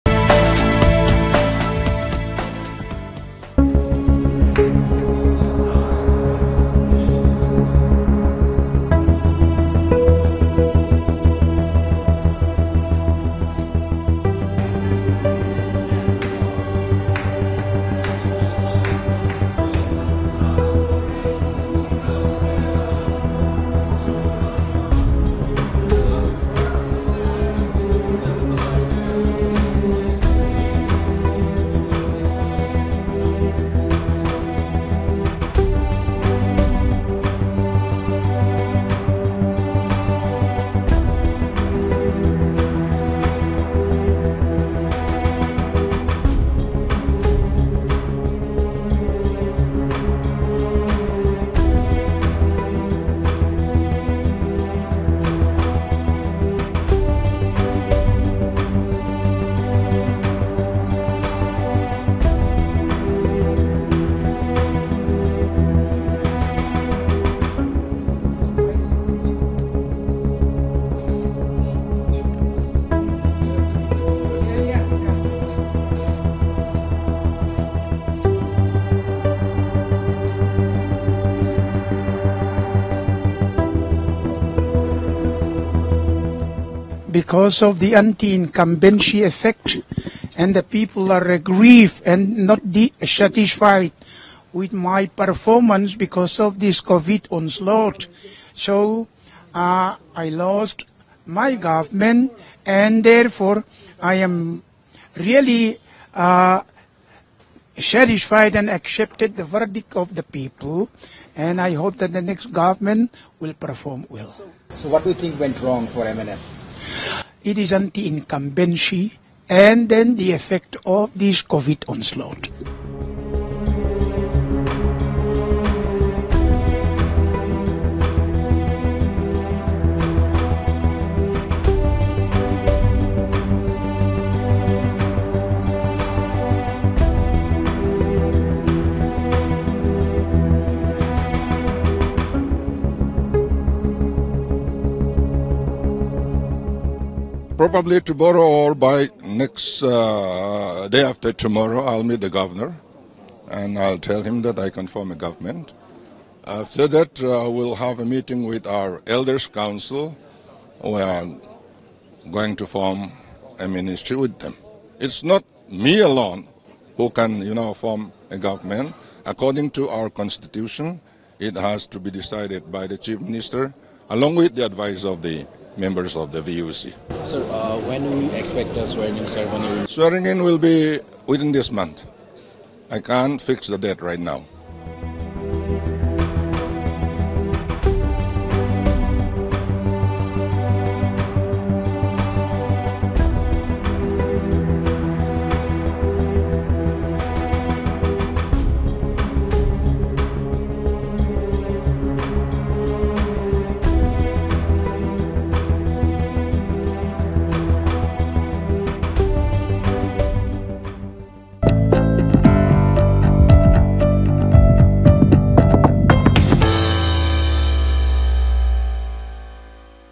Mizoram Election Results: ZPM defeats Zoramthanga’s MNF, CM candidate Lalduhoma expresses joy
The Zoram People’s Movement Party has achieved a comfortable victory in Mizoram’s Assembly Polls. The party has secured 27 seats of the total 40 seats and easily surpassed the mandatory halfway mark to form the government. The incumbent Mizo National Front has marked victory in only seven seats across the state. The outgoing Chief Minister Zoramthanga tendered his resignation to Governor Dr Hari Babu Kambhampati. Lalduhoma is the Chief Ministerial candidate of the Zoram People’s Movement Party. He registered a decisive win in the Serchhip constituency, defeating MNF candidate J Malsawmzuala Vanchhawng by 2982 votes.